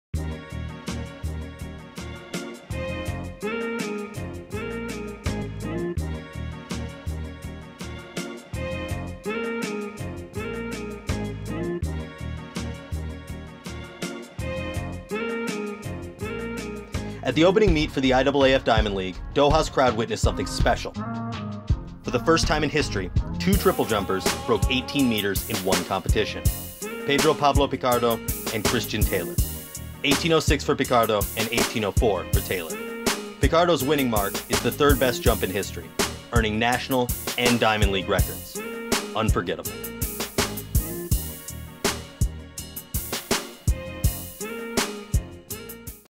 SATV+RBR Present: The Battle of Pedro Pichardo & Christian Taylor
SATV+RunBlogRun
Athlete of the Month Series: May 2015
The Battle of Pedro Pablo Pichardo & Christian Taylor
Triple Jump - IAAF Diamond League: Doha, Qatar

When Pablo Pichardo leaped 18.06 meters, he was the first man since 1996 to leap over 18 meters. When Christian Taylor responded in 18.04 meters, it was the first time in history that two Triple jumpers had gone over 18 meters. Historic.